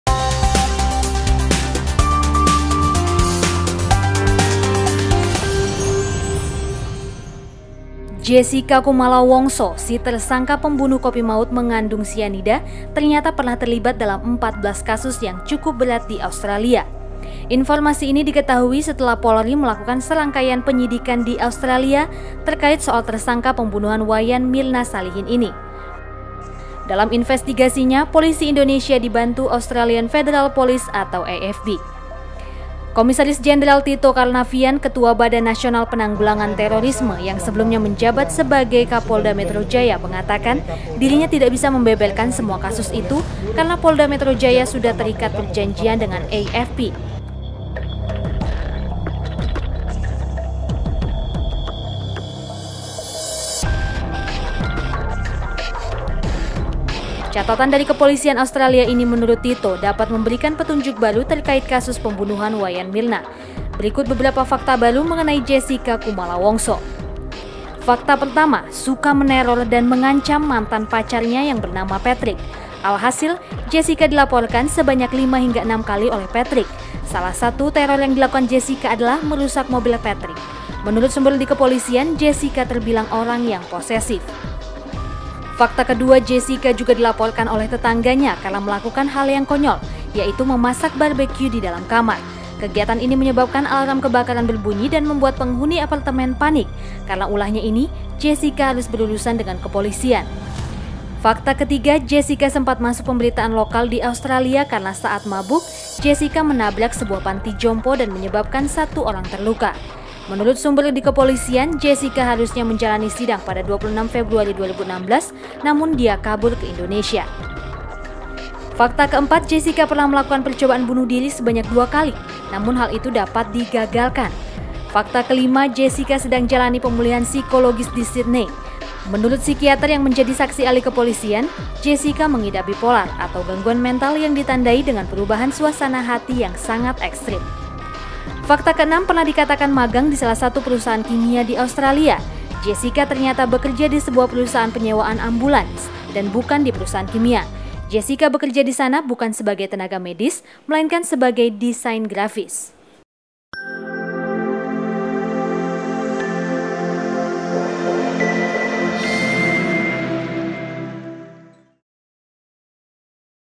Fakta Baru Tentang Jessica
Setelah bekerja sama dengan kepolisian Australia, Polda Metro Jaya mendapat 14 catatan kasus Jessica di Australia.